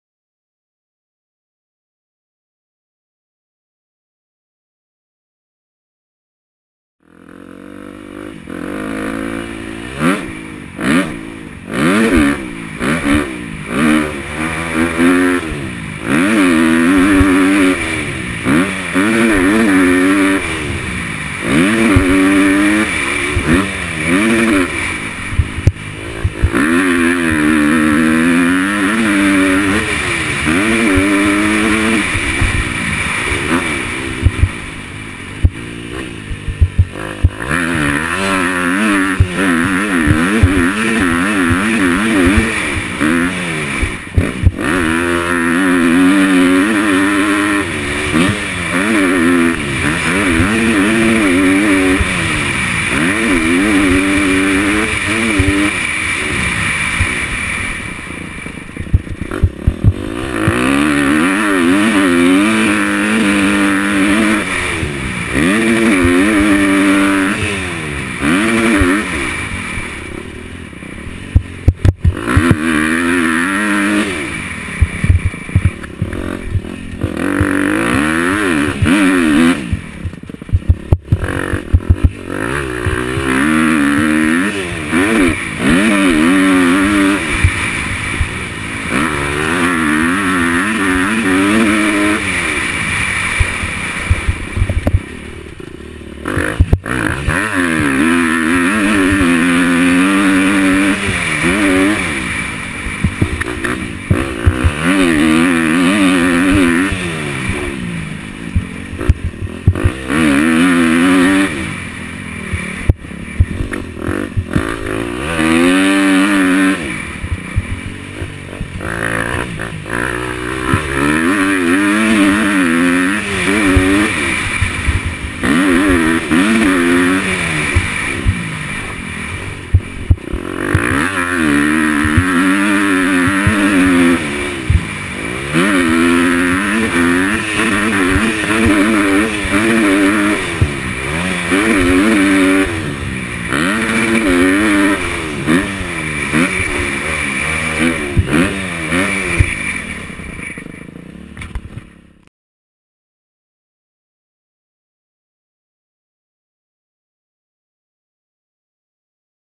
David Buller Helmet Camera 8-13-12
David Buller practicing for Unadilla 8-13-12